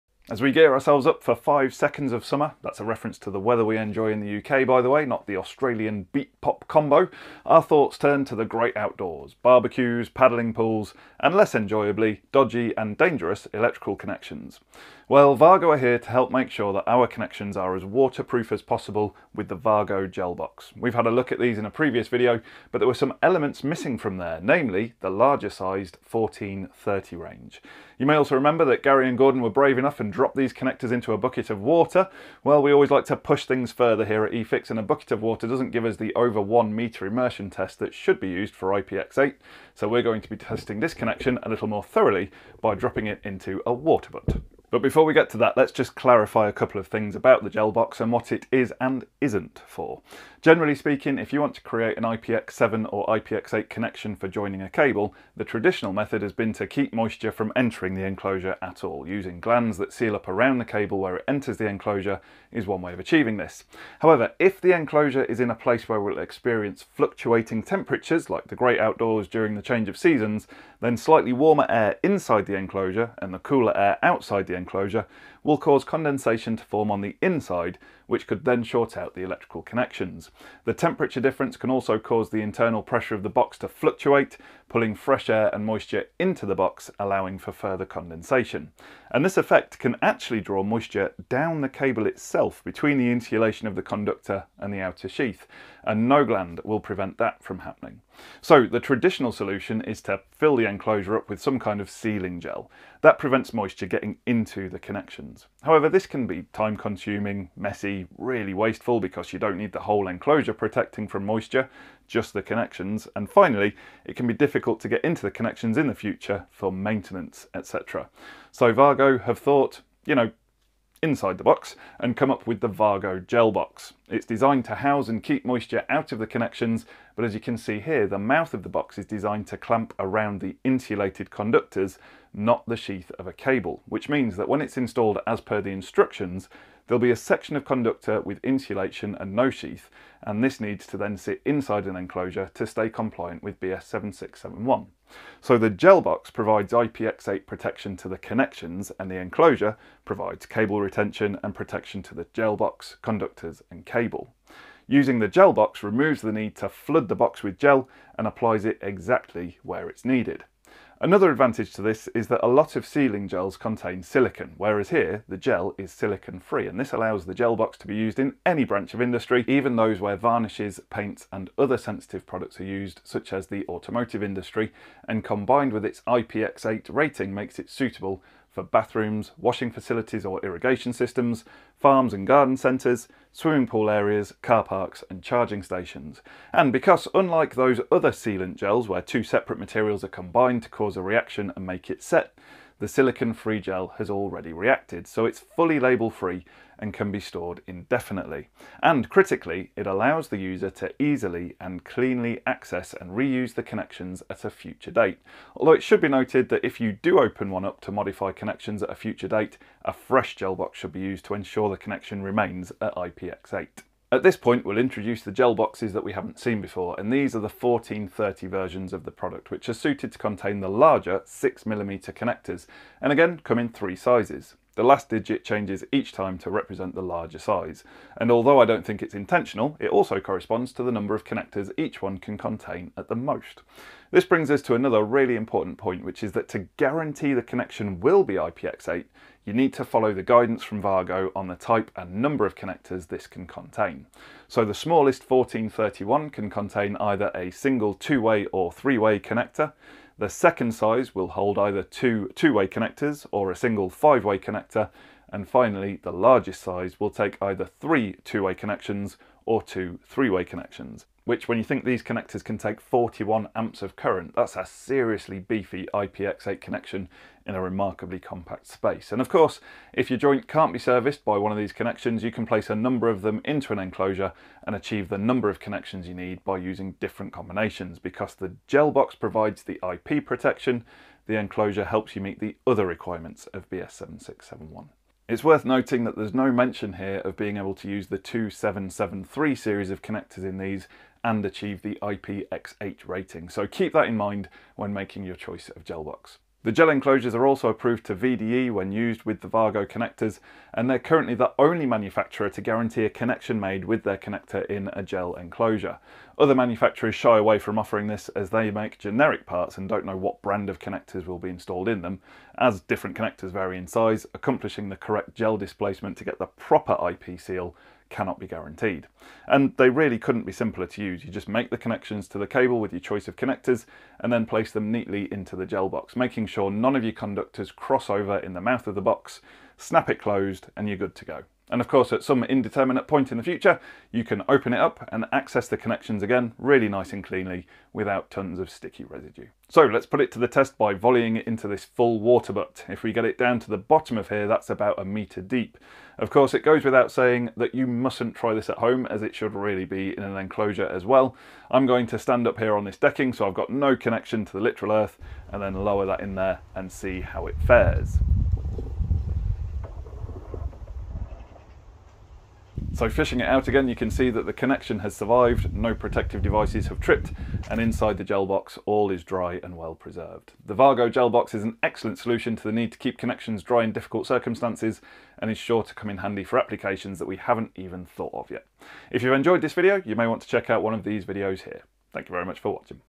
How to Make Your WAGO 221 Connectors Waterproof
The Wago Gelbox range has now been extended to include the larger Wago 221 connectors.
In this video, we show how a waterproof electrical connection can be made using the Wago Gel box. What are the advantages compared to resin joints and traditional electrical junction boxes?

== AD
Wago Gelbox

00:00 Wago Gelbox, including the larger 1430 series.
00:50 Using the Gelbox to create a waterproof electrical connection.
02:10 Enclosure inside an enclosure
02:45 Silicon free encapsulant
03:15 Re-enterable design
03:55 Gelbox 221 connector compatibility
05:00 Beware of other gel-type boxes
05:40 Does the Wago Gel box work?
Follow us on your other favourite social channels.
eFIXX is powered by you! - get involved in the channel
📻 Tune in to the latest electrical industry news podcast